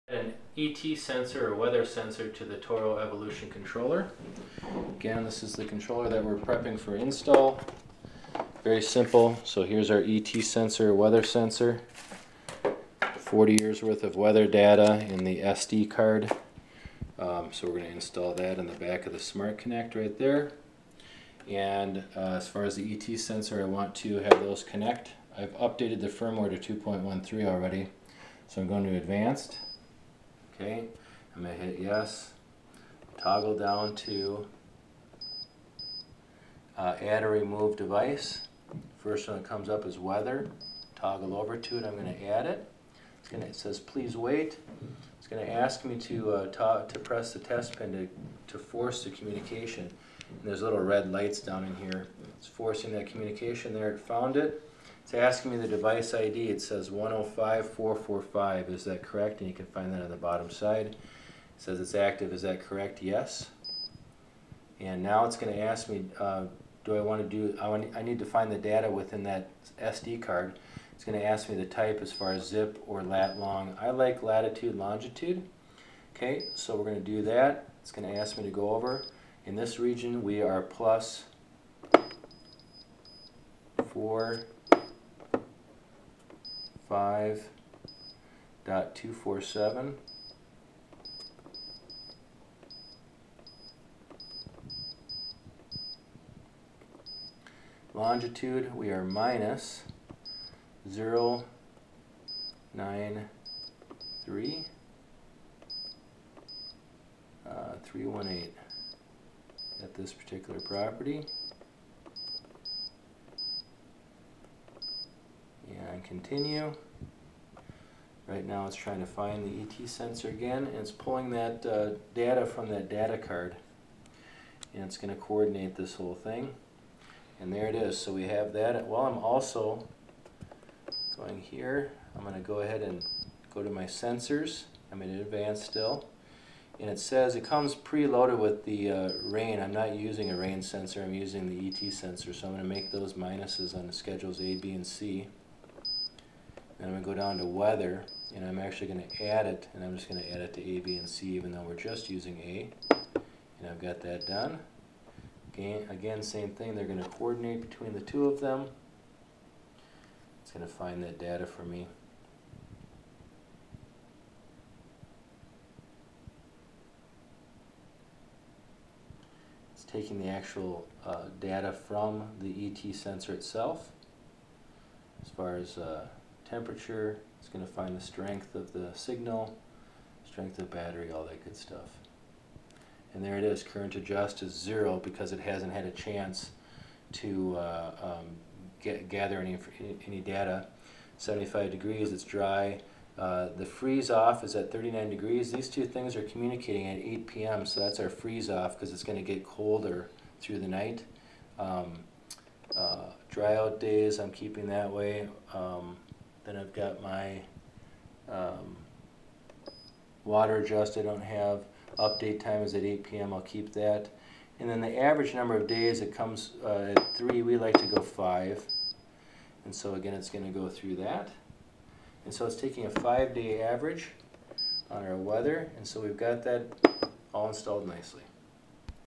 Adding an ET weather sensor to the Toro EVOLUTION® controller
It’s easy to add the Toro ET Weather Sensor to the Toro EVOLUTION® controller.  The wireless Toro ET Weather Sensor is a critical device to add to your Toro EVOLUTION® controller and will take over your built-in seasonal adjust mode and communicate daily to the controller what the actual weather conditions are at YOUR site.  The extensive algorithm discerns how the actual weather is deviating from the 40 years of historical ET data from that latitude and longitude and will adjust the watering times and frequency up or down as needed.  It delivers the right amount of water at the right times and saves you 40 – 60 percent on your water bill.  View this video to simply add the Toro ET Weather Sensor.